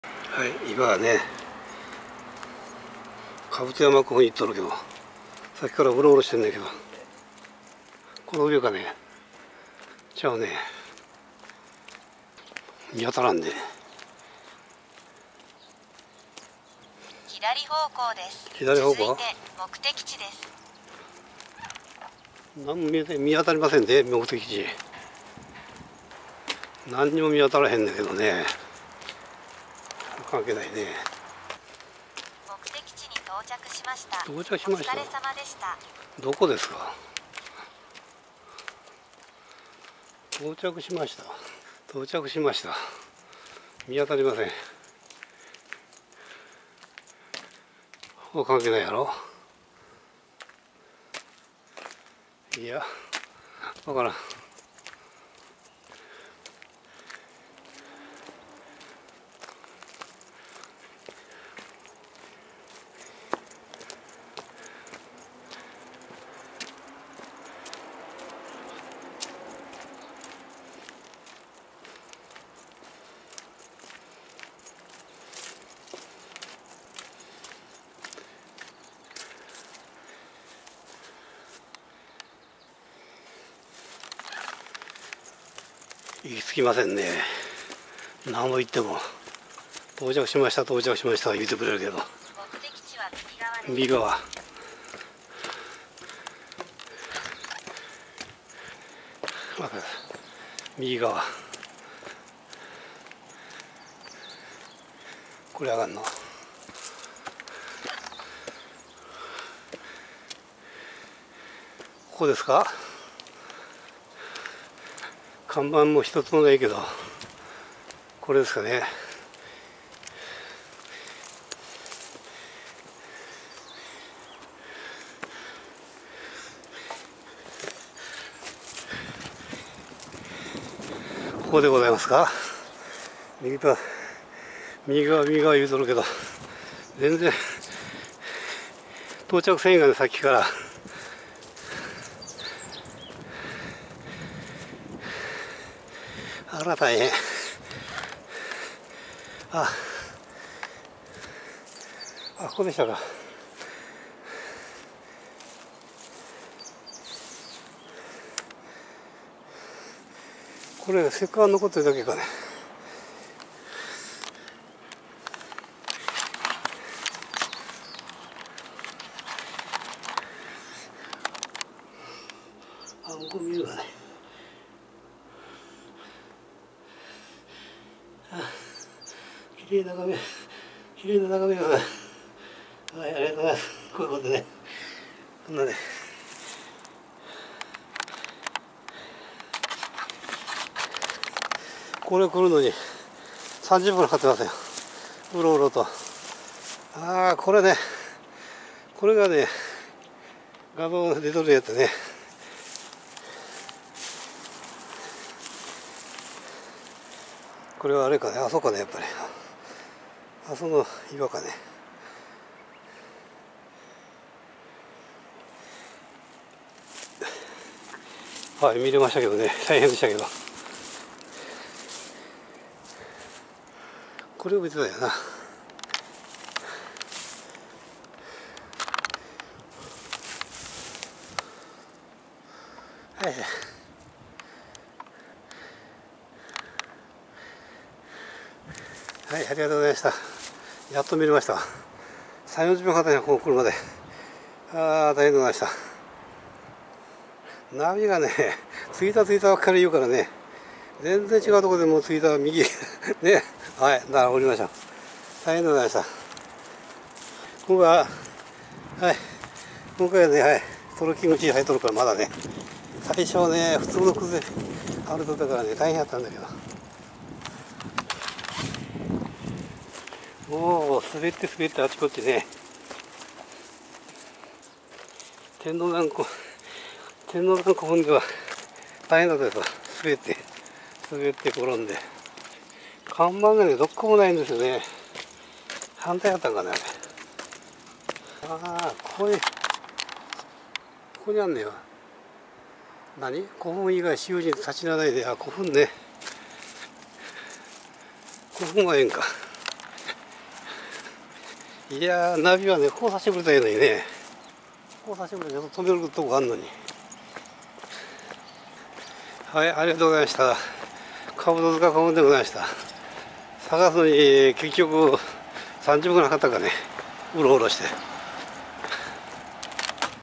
兜塚古墳 前方後円墳40ｍ高さ3.5ｍ 車のナビとスマホのナビとで 位置確認しながら 近ずくが見当たりません いつものこの有様です。30分以上近くをウロウロ・・
兜塚古墳は寺川右岸の台地上に存在している全長40m、後円部径20m、高さ3.5m、前方部幅19m、高さ3.5mの前方後円墳です。上の画像は斜面下から撮影したもので中央が前方部、右奥に向けて後円部が存在しています。
墳丘の存在する丘陵を削って東西48m、南北24～26mほどの方形の土台が形成され